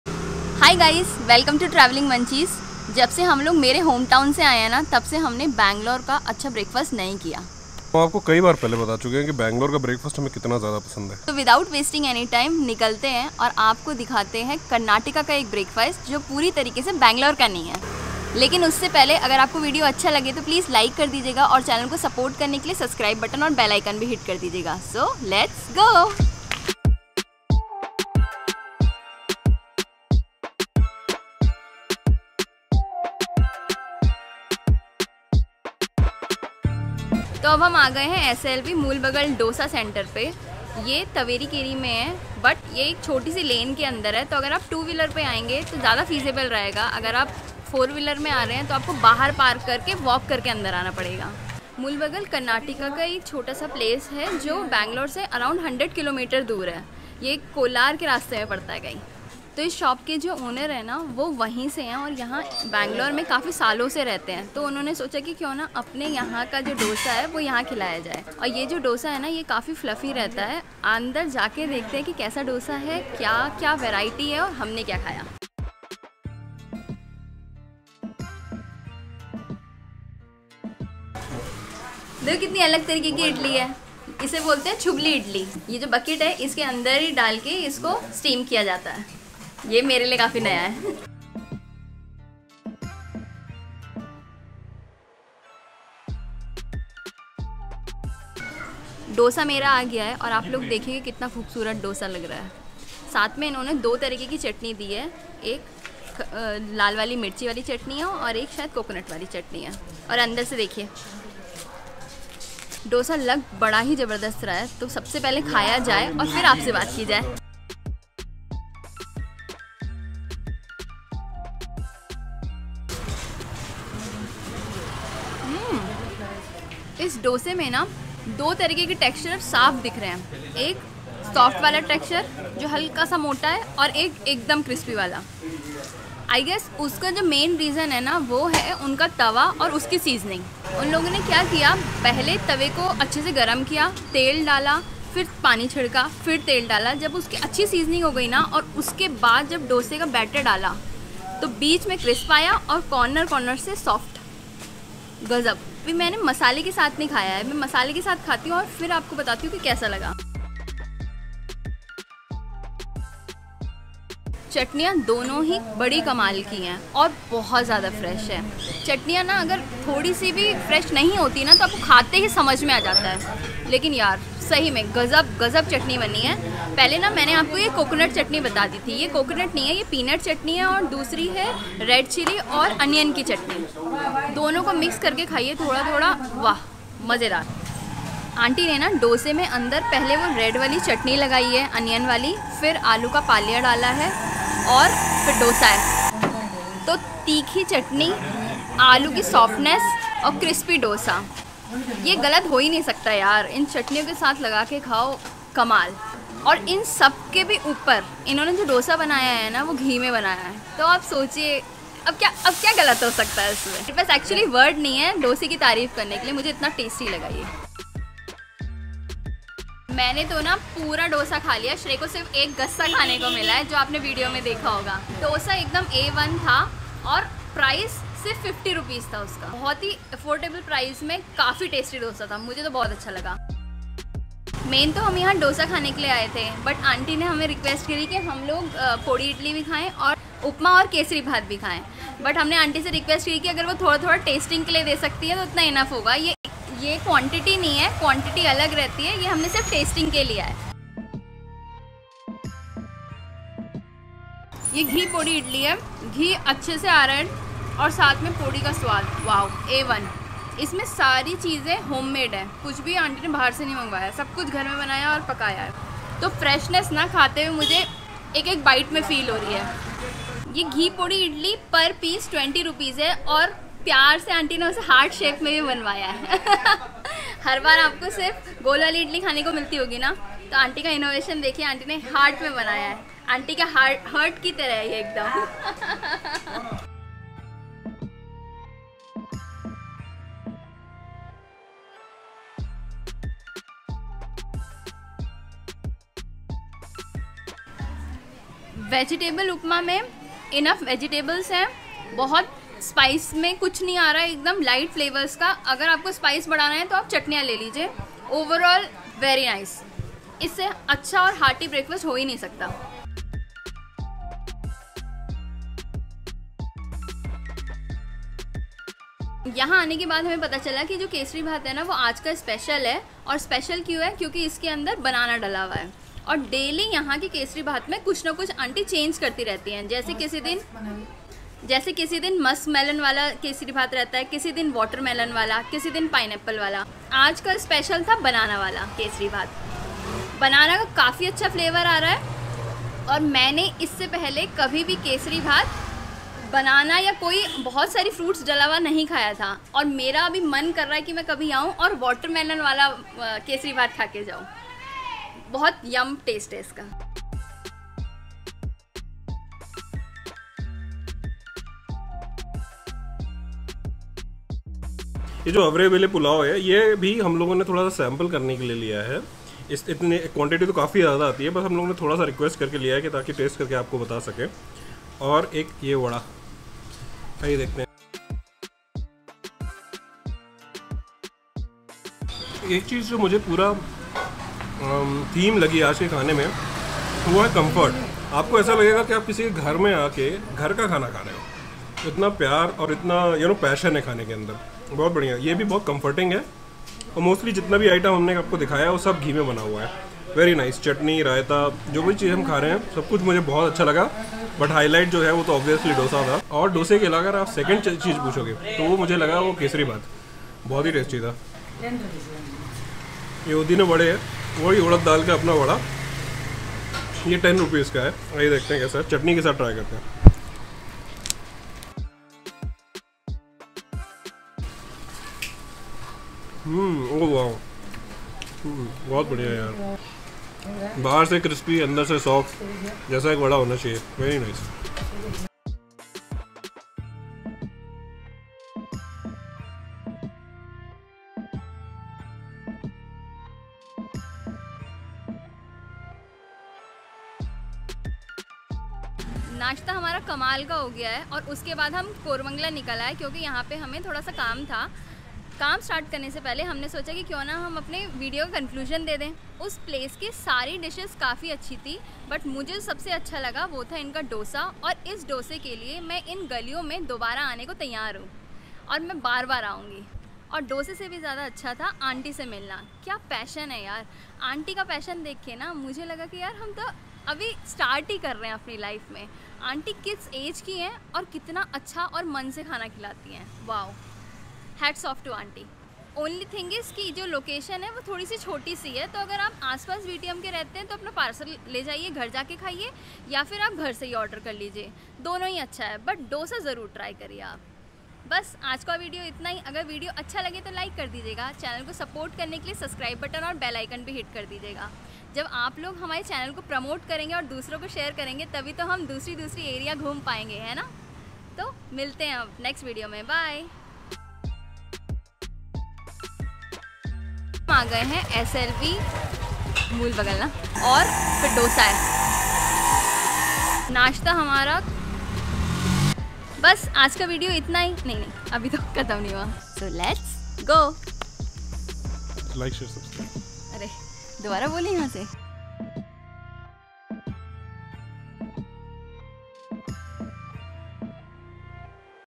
Mulbagal Dosa in Bangalore | best Breakfast in Bangalore | Bangalore food tour | Budget Friendly
S.L.V. Mulbagal Dosa

Mulbagal Dose: Rs. 50/-
Ghee Pudi Idli : Rs. 20/-
Masala bath : Rs. 30/-
Kesari Bath: Rs. 30/-
Vade: Rs. 10/-

Mulabagilu is a town and taluk headquarters of Mulabagilu taluk in the Kolar district in the state of Karnataka, India. It lies just off the National Highway 75 as the easternmost town of the state and a hill landmark.

0:00 - Introduction
0:37 - Reached Location
1:38 - Mulbagal Dose
3:53 - What Else?
4:10 - Ghee Pudi Idli
4:51 - Chow Chow Bath
6:02 - Avrebelle Pulao
6:52 - Vade
7:29 - Conclusion
8:35 - Bloopers